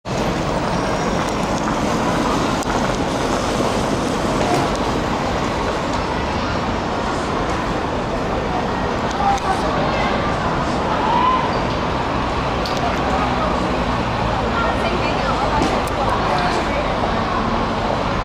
中國式橫過馬路 鄰近地區很羨慕 包容吧! 被蠶食的社會 19/12/2014
我們熟悉的城市正步向死亡, 公民教育被蠶食, 被自由行人士文化溝淡, 再沒有人依從過路設施, 此處上面就有行人天橋NF296, 但是自由行人士不行天橋, 人車爭路, 險象環生,包容吧!你懂的.
上水龍琛路
2014年12月19日15:33
374G 章《道路交通(交通管制)規例》：39條：
(a)任何人不得在以下情況橫過馬路─
(iii) 身處距離行人天橋或行人隧道或該天橋或隧道的任何部分15米範圍內，但卻不使用該天橋­­或隧道；
即屬犯罪，可處罰款$2000。